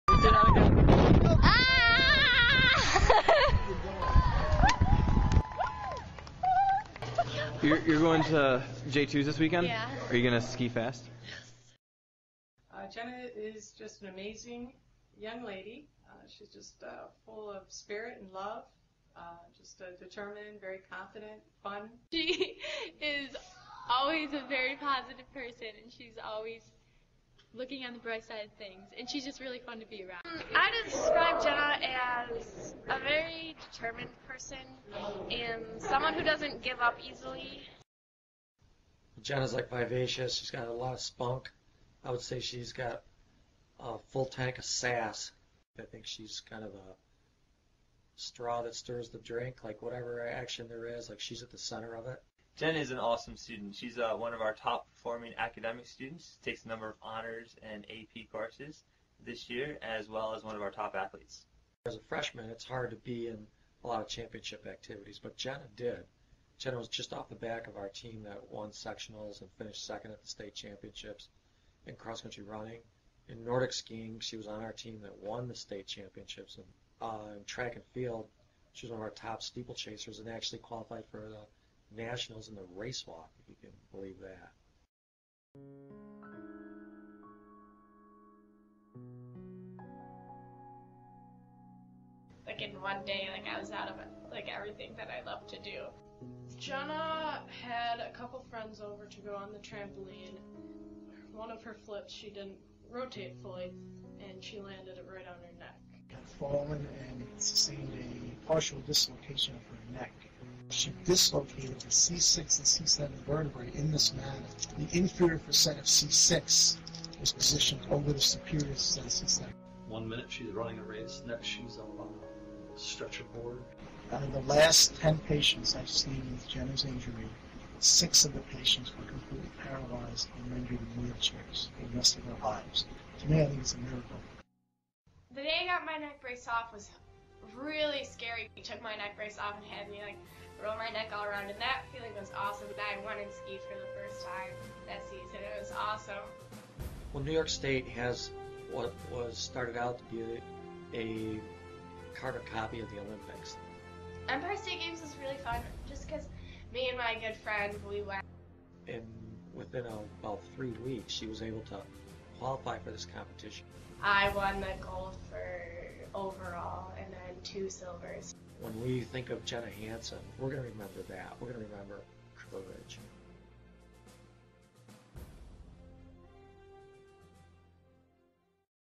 The Jenna Hanson Story
On October 17, 2010, Jenna Hanson broke her neck on a trampoline. She was completely immobilized in the hospital for days before the doctors released her in a permanent neck brace. She used a wheel chair to get around school, and couldn't lift anything over five pounds.

As a 3 sport varsity athlete, it was difficult for her to not spend her days training and being with her teammates. But with the guidance of Bernie Gardner, her coach, Jenna spent her months of inactivity focusing. Mental training and imagery were key motivators to her recovery.

To everyone's surprise, In February 2011, Jenna was cleared by the doctors to participate in sports. Just four months after dislocating and fracturing her C6 and C7 vertebrae, Jenna won a gold medal and two silver medals at the Empire State Games for Nordic Skiing.

An inspiration to us all, Jenna's story reminds us of how important perseverance is in overcoming adversity.